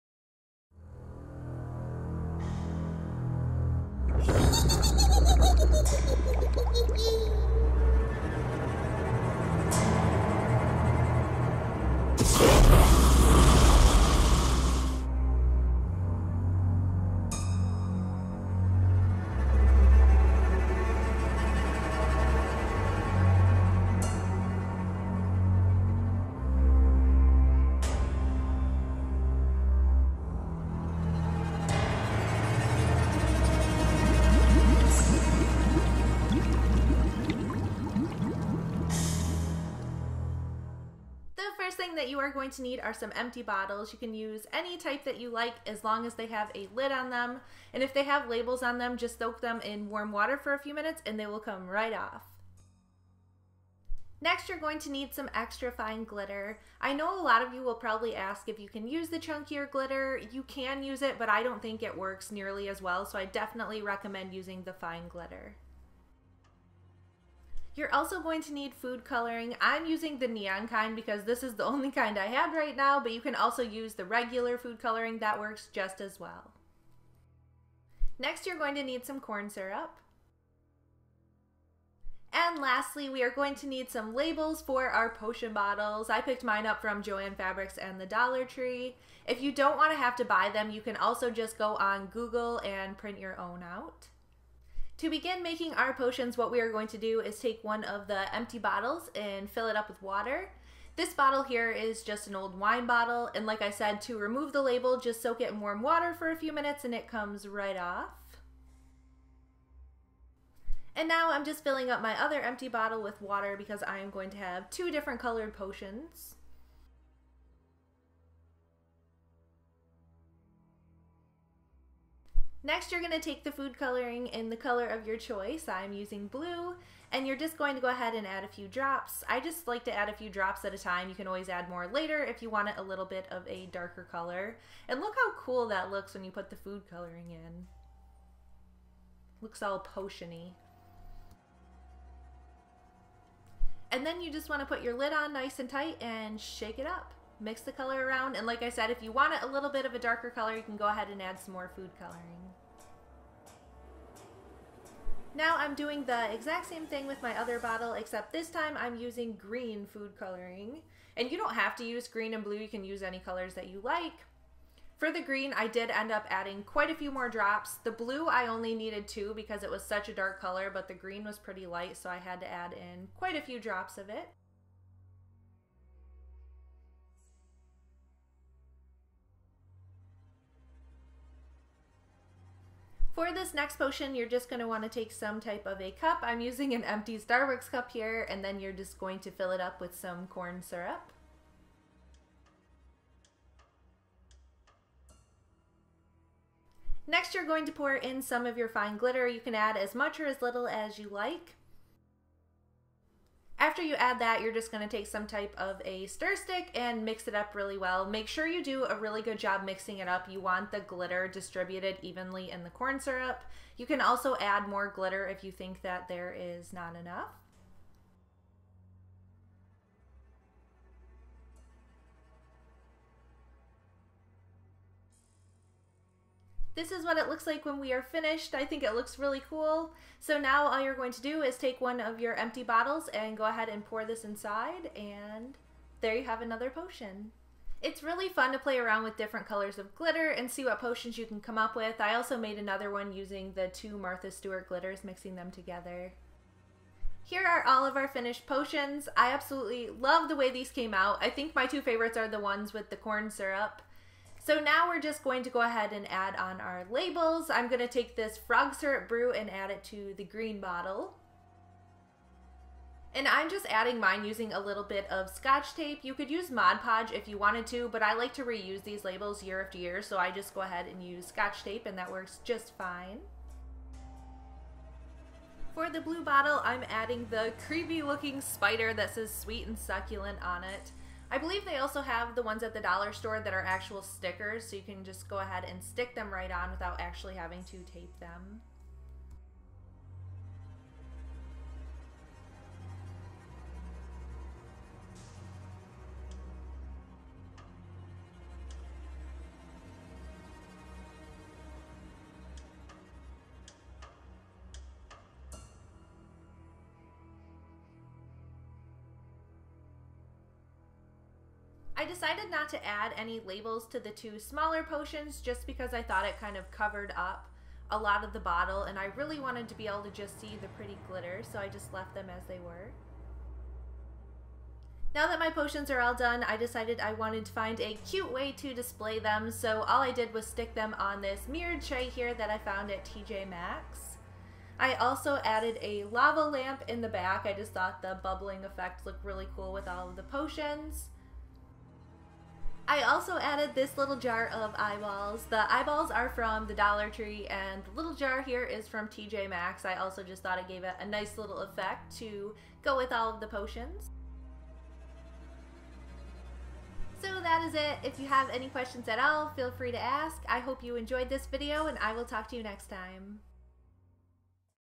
DIY Halloween Potion Bottles - Halloween Room Decor
It's almost Halloween:) Today I"m showing you how to make DIY Halloween potion bottles. These are fun and easy to make.